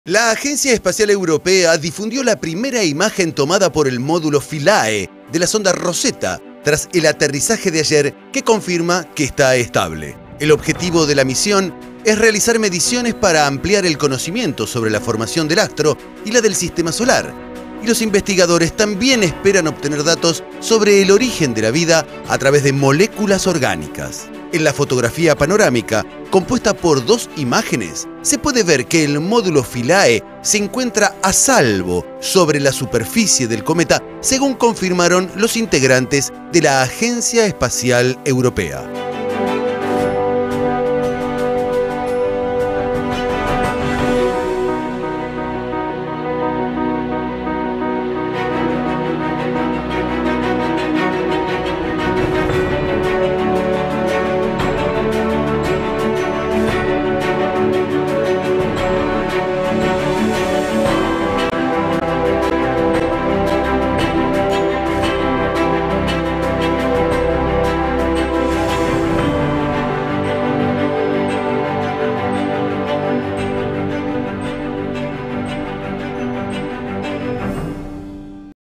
El módulo Philae envió la primera foto desde la superficie del cometa
La Agencia Espacial Europea (ESA) difundió la primera imagen tomada por el módulo Philae, de la sonda Rosetta, tras el aterrizaje de ayer, que confirma que está estable.

En la fotografía panorámica, compuesta por dos imágenes, se puede ver que el módulo Philae se encuentra a salvo sobre la superficie del cometa 67/P Churyumov-Gerasimenko, según confirmó ESA.

El jefe del Departamento de Ingeniería de Sistemas de tierra de la agencia espacial, Juan Miró, calificó de "muy bueno" el aterrizaje, "perfecto desde el punto de vista de la trayectoria", según informó a la agencia EFE.

Si bien la comunicación con el módulo Philae se mantiene y se encuentra estable, los responsables de la misión aun deben verificar si recibe suficiente energía solar para continuar su actividad, sostuvo Miró.

Durante el aterrizaje, los dos arpones del módulo no se activaron y eso provocó que rebotara dos veces sobre la superficie.

Además, un sistema de descenso compuesto por un depósito de gas frío a mucha presión que tenía que salir hacia arriba y presionar a Philae hacia el suelo, tampoco funcionó, explicó Miró.

De todas formas, los especialistas de la ESA señalaron que el sistema no era crítico y por eso el minilaboratorio (del tamaño de una heladera, con 20 instrumentos de medición a bordo) que viajó en la sonda Rosetta pudo aterrizar.

El objetivo de la misión es realizar mediciones para ampliar el conocimiento sobre la formación del astro y la del Sistema Solar, y los investigadores también esperan obtener datos sobre el origen de la vida a través de moléculas orgánicas.